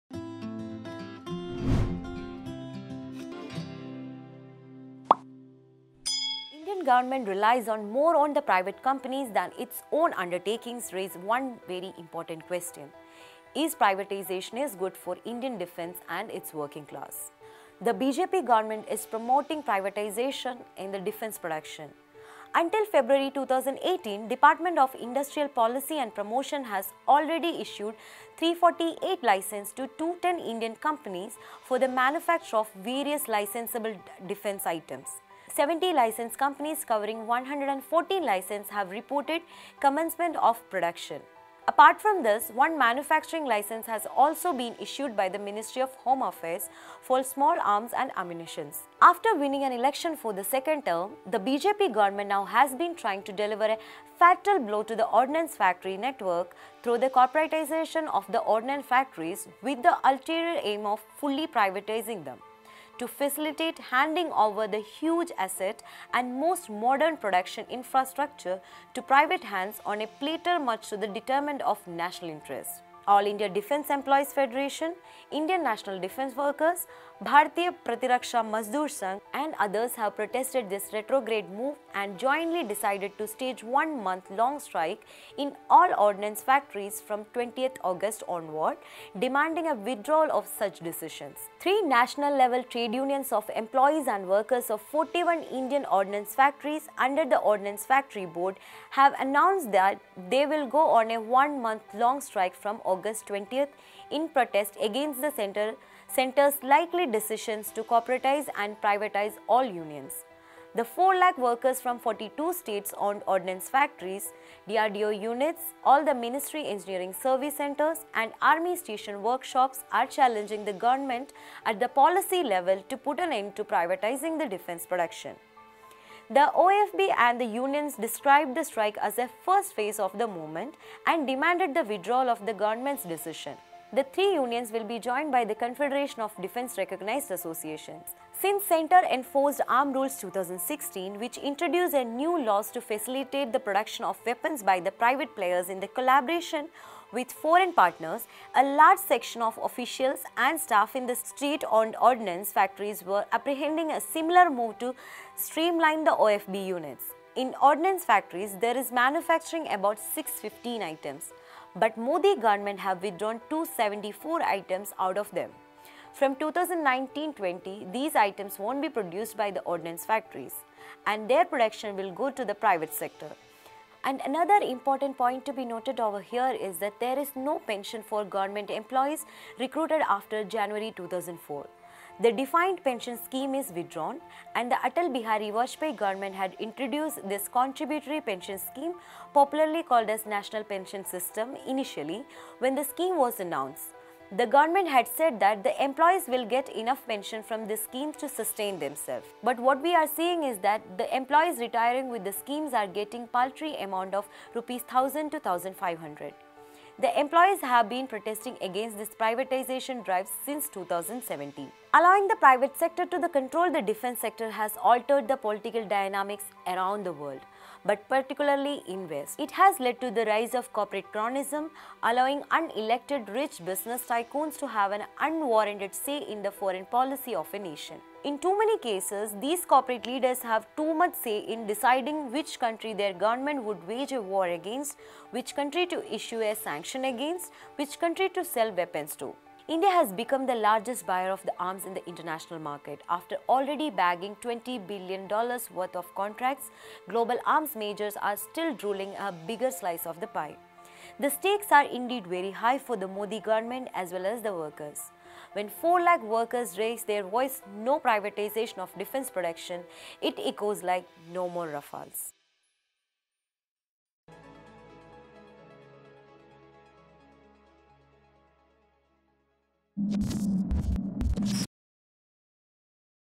Ordnance Factory Workers to Strike Against Privatisation
About 4 lakh employees of the 41 ordnance factories will be going on a month-long strike from August 20 against the Union government's move to corporatise the factories. On one hand, the Narendra Modi led BJP government promotes nationalism, while on the other hand, they are making attempts to privatise the defence sector at the cost of risking national security.